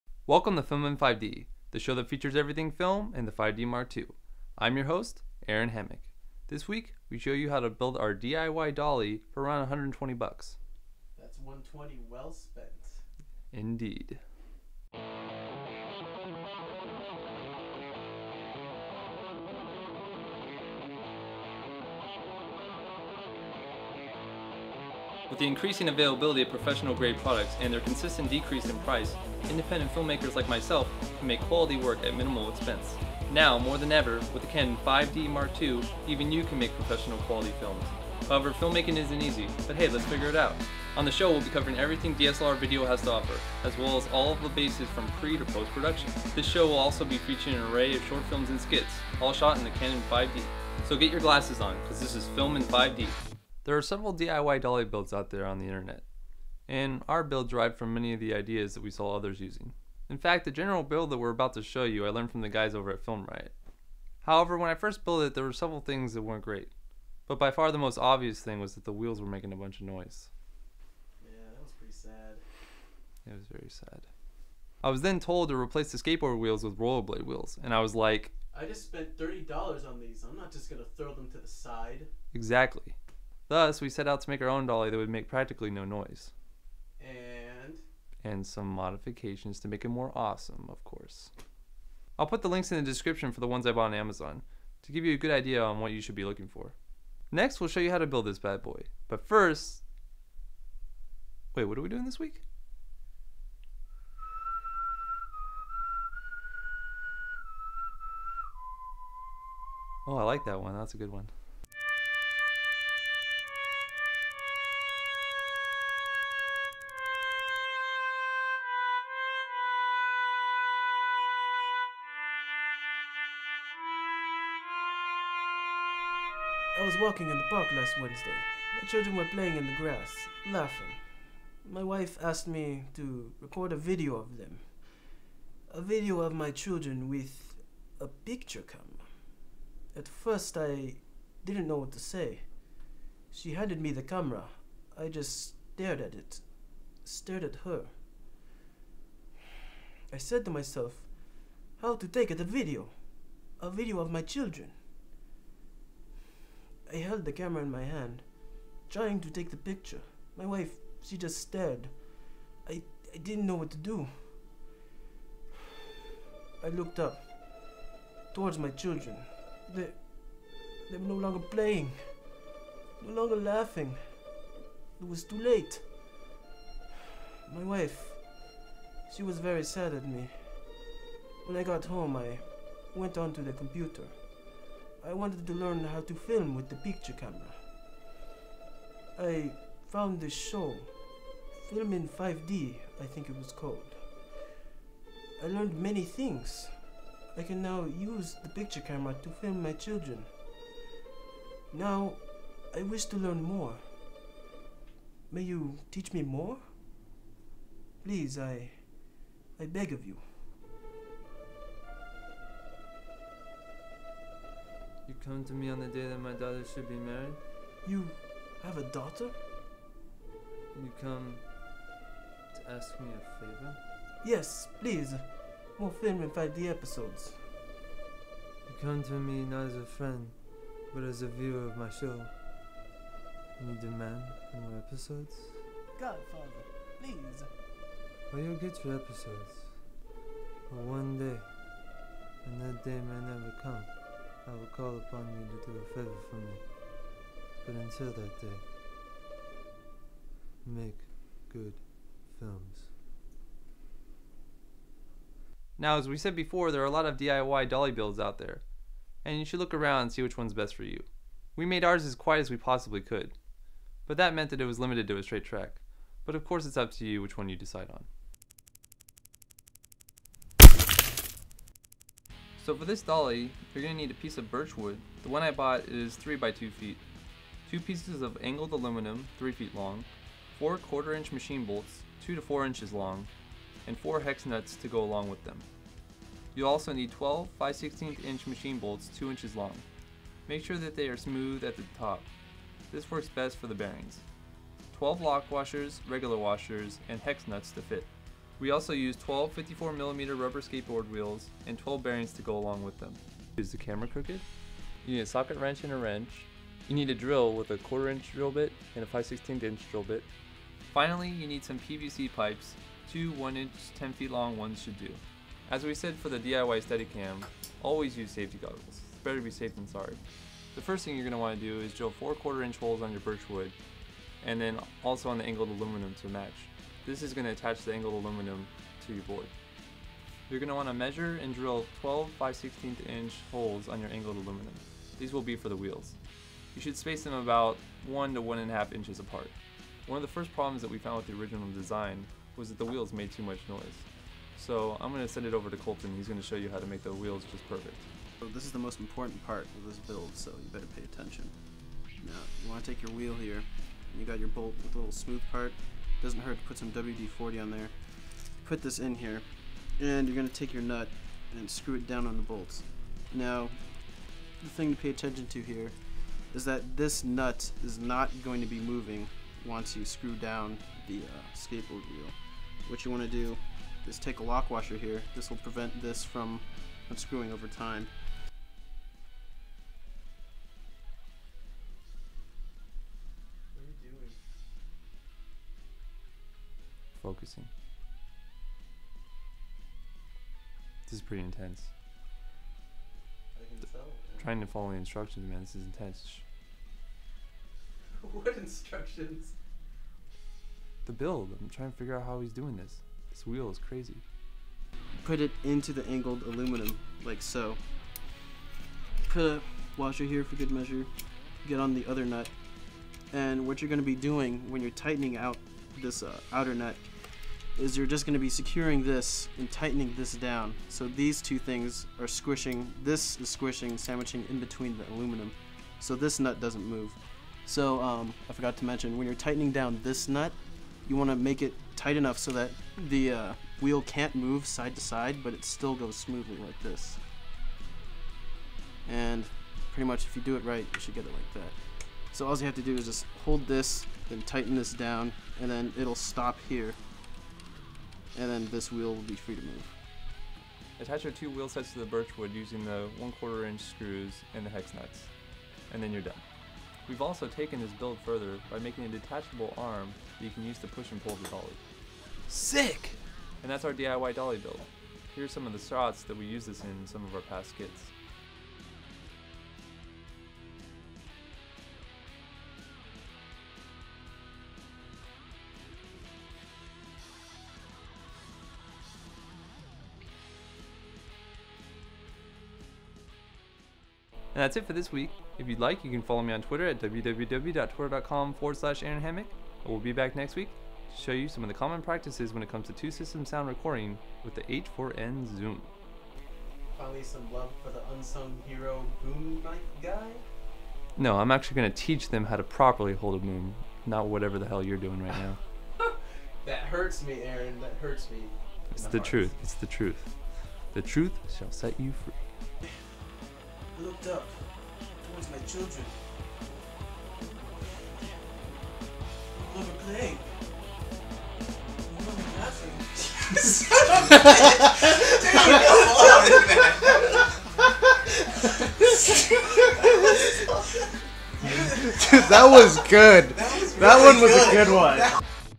FilmIn5D Episode 8: The Godfather, and how to Build a DIY Dolly for your Canon 5d mark ii
This week we show you how to build a DIY dolly for under 150 dollars.

SHOPPING LIST:

1 Birch wood (3x2)'
2 Angled Aluminum (3')
4 (1/4") carriage bolts (4" long)
4 (1/4") hex nuts
12 (5/16") machine bolts (2" long)
12 lock washers
12 regular washer
24 (5/16") hex nuts
12 skateboard wheels
24 bearings

LINKS:

Skateboard wheels

Questions?  Send them to us on twitter!